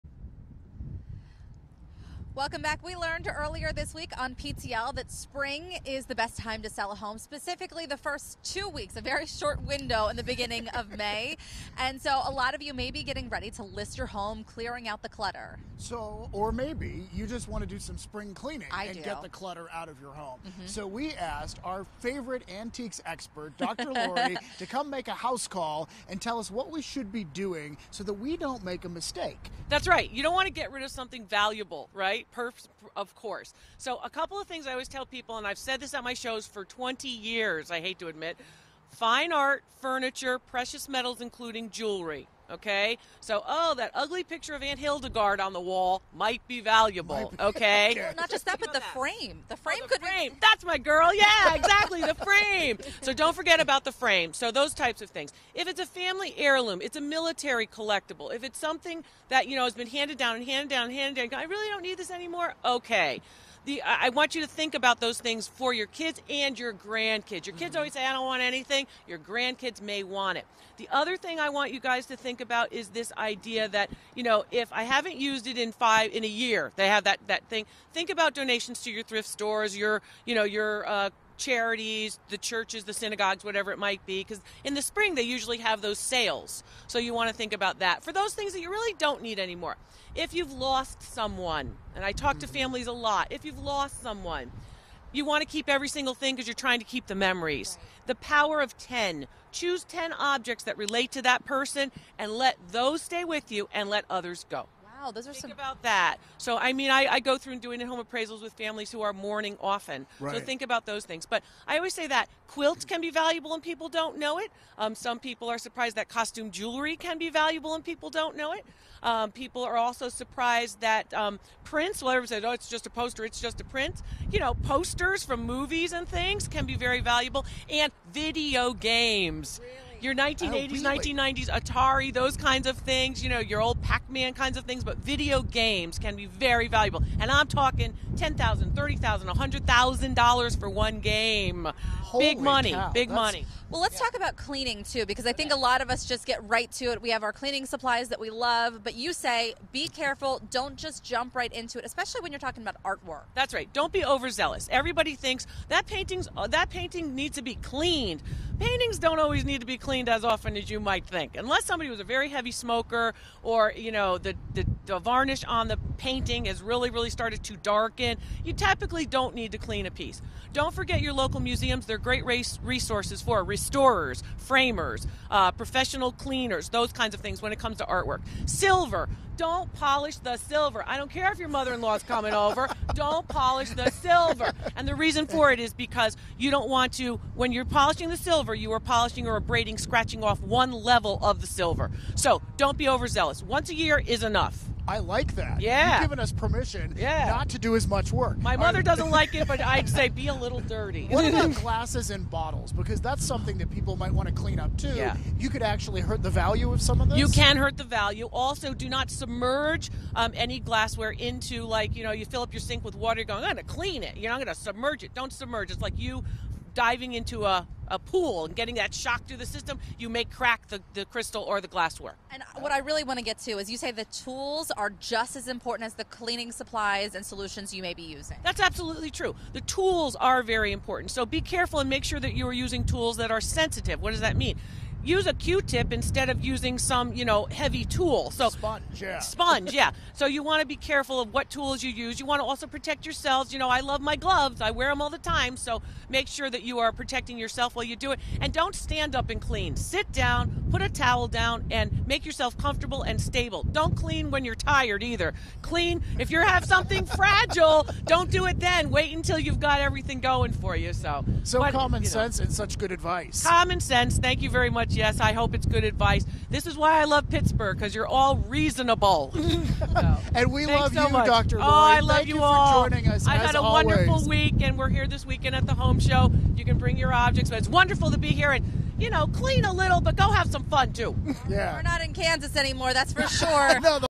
Spring Cleaning With Dr. Lori
Antiques appraisal expert Dr. Lori has some tips on de-cluttering your house for springtime!